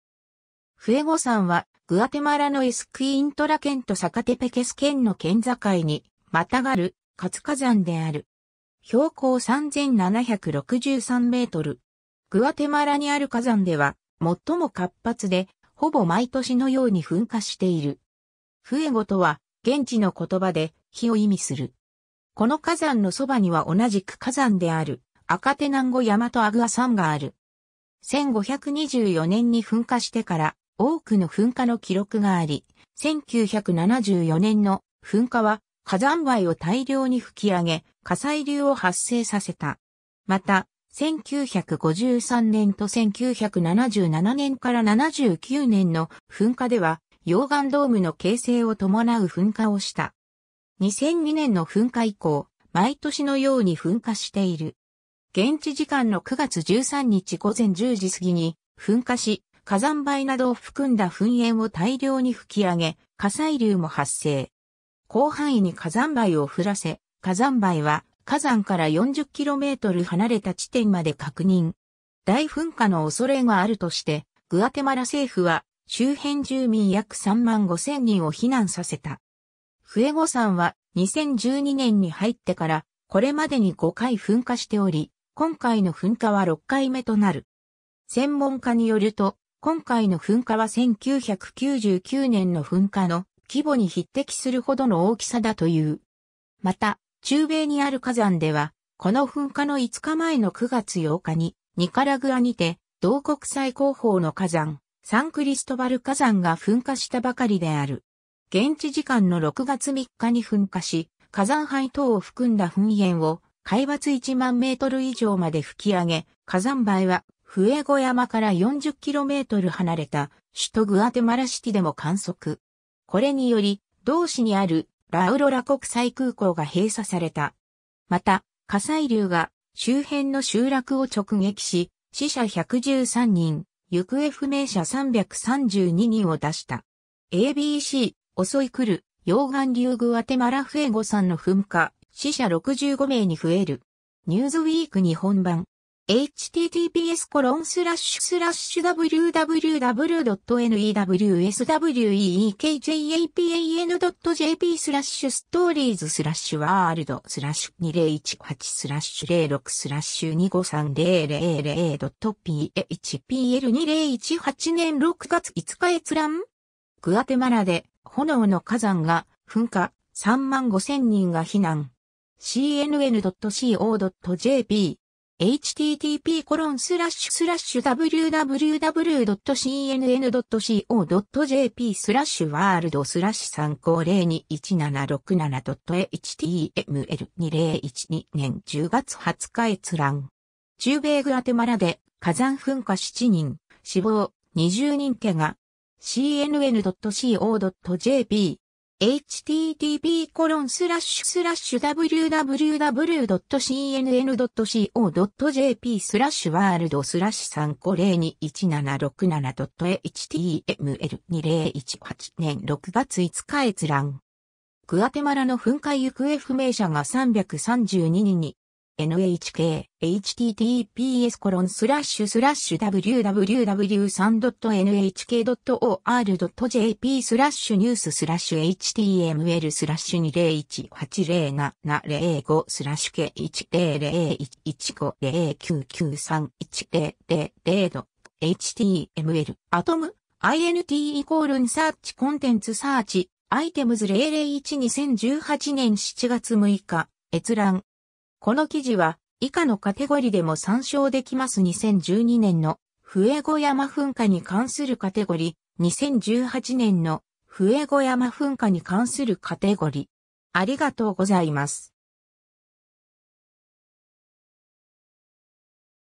フエゴ山
フエゴ山（フエゴさん、スペイン語: Volcán de Fuego）は、グアテマラのエスクィントラ県とサカテペケス県の県境にまたがる活火山である。
標高3,763m。
 グアテマラにある火山では最も活発で、ほぼ毎年のように噴火している。
「フエゴ」とは、現地の言葉（スペイン語）で「火」（または「炎」）を意味する。
この火山のそばには同じく火山であるアカテナンゴ山とアグア山がある。
 1524年に噴火してから、多くの噴火の記録があり、1974年の噴火は火山灰を大量に噴き上げ、火砕流を発生させた。
また、1953年と1977年-79年の噴火では、溶岩ドームの形成を伴う噴火をした。
2002年の噴火以降、毎年のように噴火している。
 現地時間の9月13日午前10時過ぎに噴火し、火山灰などを含んだ噴煙を大量に噴き上げ、火砕流も発生。
広範囲に火山灰を降らせ、火山灰は火山から40km離れた地点まで確認。
大噴火の恐れがあるとして、グアテマラ政府は周辺住民約35,000人を避難させた。
フエゴ山は、2012年に入ってからこれまでに5回噴火しており、今回の噴火は6回目となる。
専門家によると今回の噴火は1999年の噴火の規模に匹敵するほどの大きさだという。
 また、中米にある火山では、この噴火の5日前の9月8日にニカラグアにて同国最高峰の火山、サンクリストバル火山が噴火したばかりである。
 現地時間の6月3日に噴火し、火山灰等を含んだ噴煙を海抜10,000m以上まで吹き上げ 、火山灰はフエゴ山から40km離れた首都グアテマラシティでも観測。
これにより同市にあるラ・アウロラ国際空港が閉鎖された。
また、火砕流が周辺の集落を直撃し、死者113人、行方不明者332人を出した。
^ a b c “襲い来る溶岩流　グアテマラ・フエゴ山の噴火、死者65名に増える”. ニューズウィーク日本版. (2018年6月5日). 2018年6月5日閲覧。
^ “グアテマラで「炎の火山」が噴火、3万5000人が避難”. CNN.co.jp. (2012年9月14日). 2012年10月20日閲覧。
^ “中米グアテマラで火山噴火　７人死亡、２０人けが”. CNN.co.jp. (2018年6月4日). 2018年6月5日閲覧。
^ “グアテマラの噴火 行方不明者が332人に”. NHK. (2018年7月5日). 2018年7月6日閲覧。
 この記事は以下のカテゴリでも参照できます 2012年のフエゴ山噴火に関するカテゴリ: 2018年のフエゴ山噴火に関するカテゴリ: